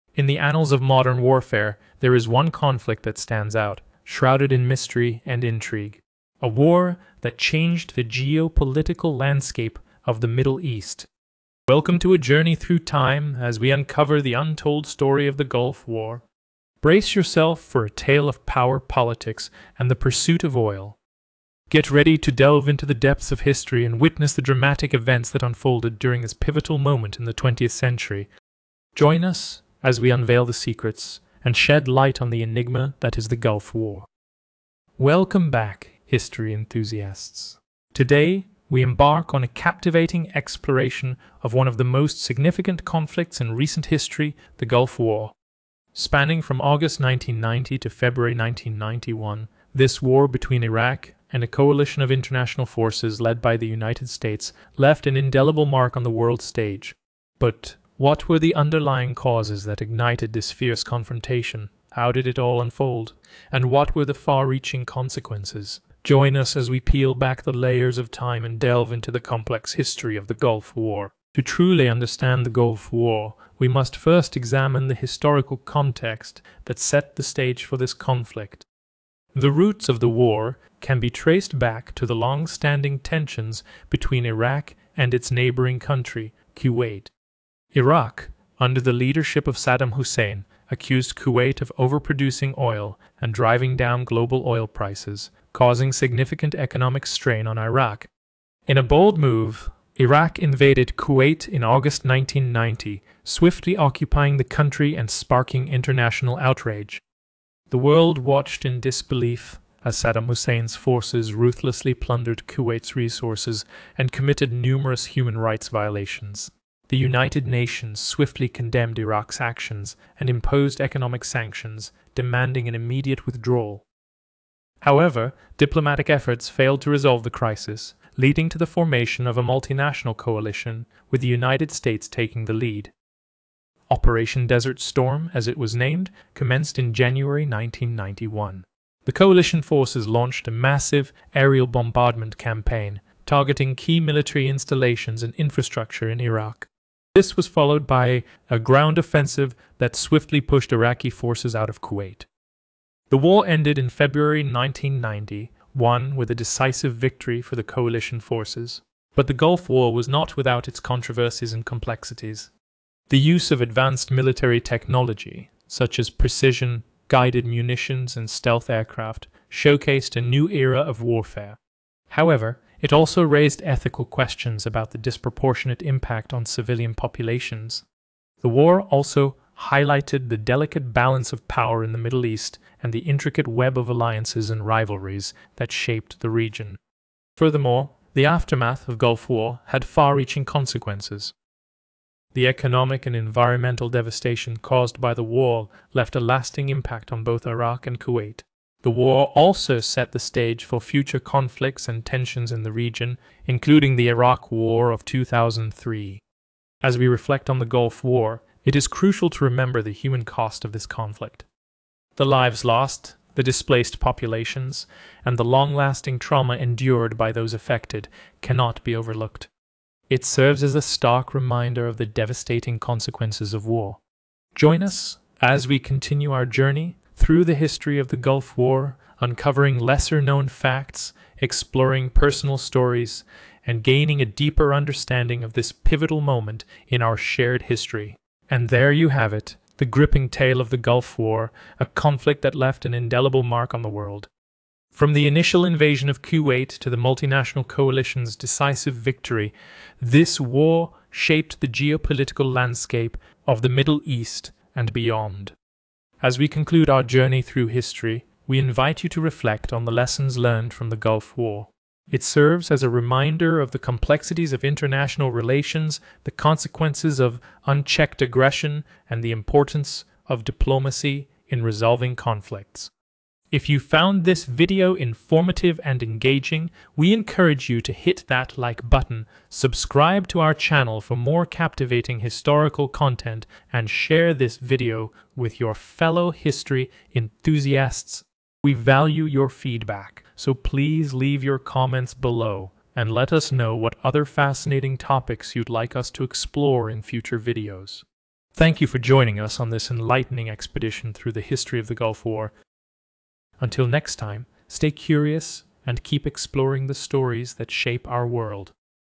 The History of the Gulf War
Contents:
00:11 - Chapter 1: The Untold Story of the Gulf War
01:26 - Chapter 2: The Causes and Invasion of Kuwait
02:03 - Chapter 3: International Response and Operation Desert Storm
02:40 - Chapter 4: A New Era of Warfare and Complexities
03:09 - Chapter 5: Consequences and Aftermath
04:12 - Chapter 6: Reflection and Lessons Learned

Discover the untold stories and pivotal moments that shaped the Gulf War in this captivating journey through history. From the origins of the conflict to its dramatic conclusion, this video provides a comprehensive and gripping account that will leave you craving for more. Join us as we delve into the complexities and significance of one of the most defining wars of our time.